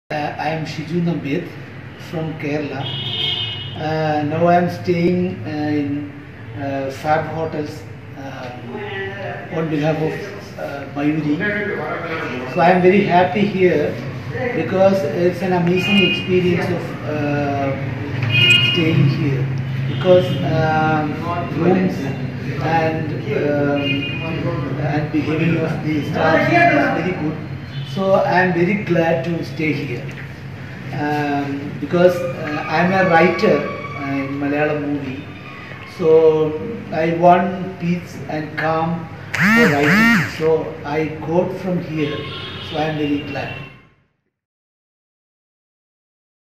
Shiju at FabHotel Mayuri Residency, Bangalore | Guest Review
Shiju talks about his experience of staying at FabHotel Mayuri Residency, Bangalore.